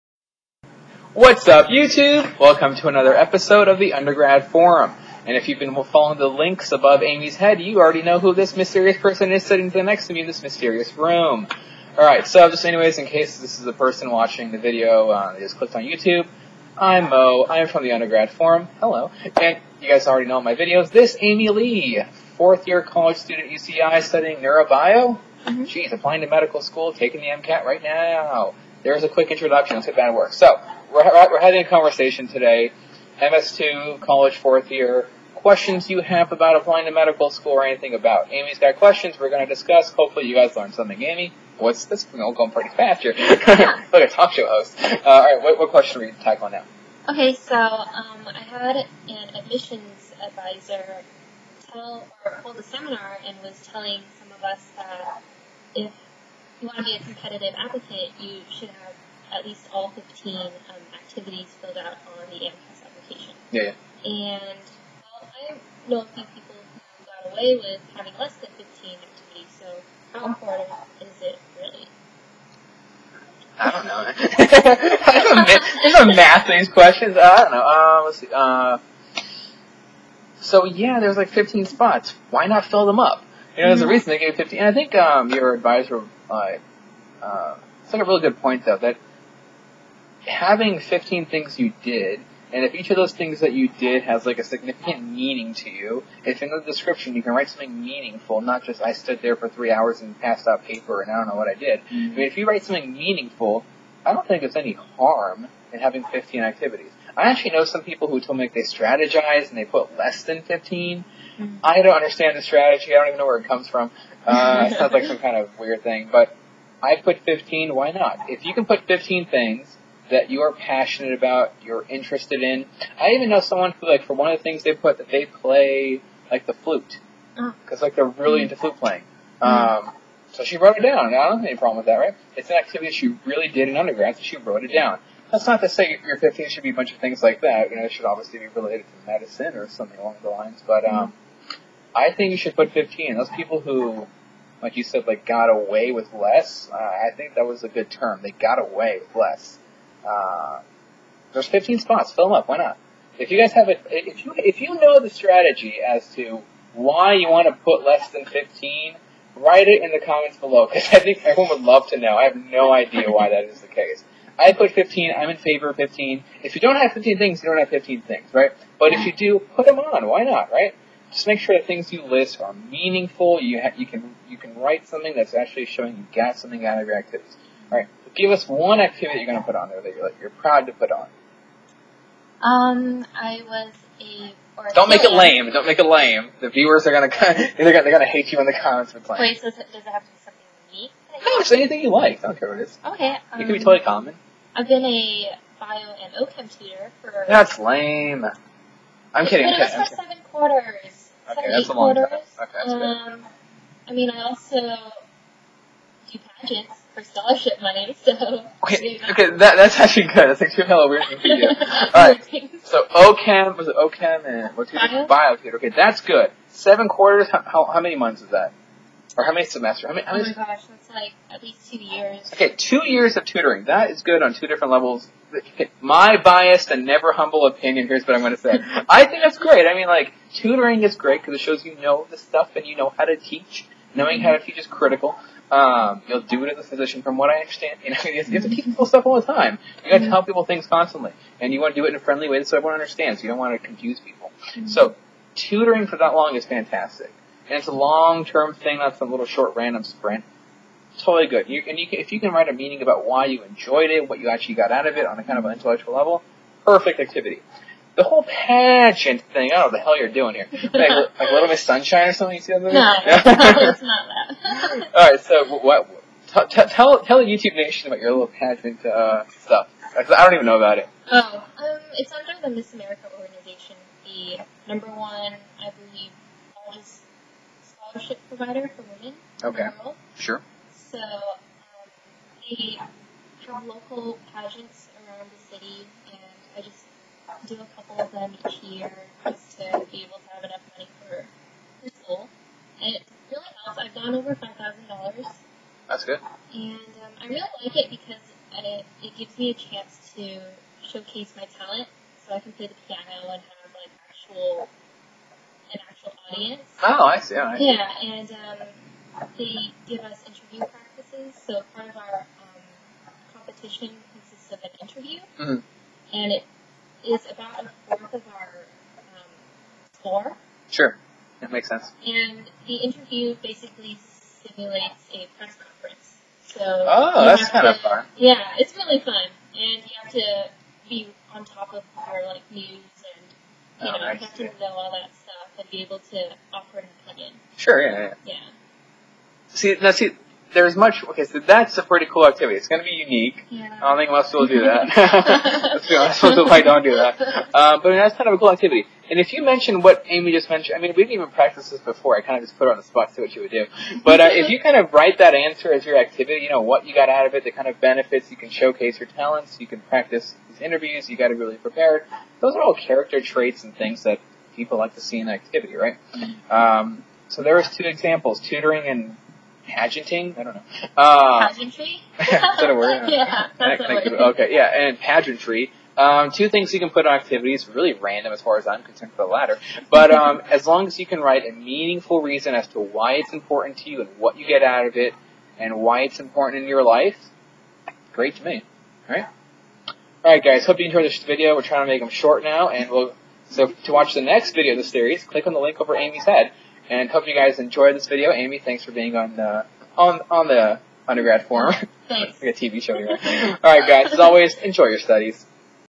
The 15 Activities on AMCAS - Conversation between a Medical Student and an Undergraduate
This is a video concerning the 15 Activities on AMCAS. This is a conversation between a Medical Student (me!) and an Undergraduate.

1) DocOssareh Official Recommended Resource List

5) Audible Free Audiobook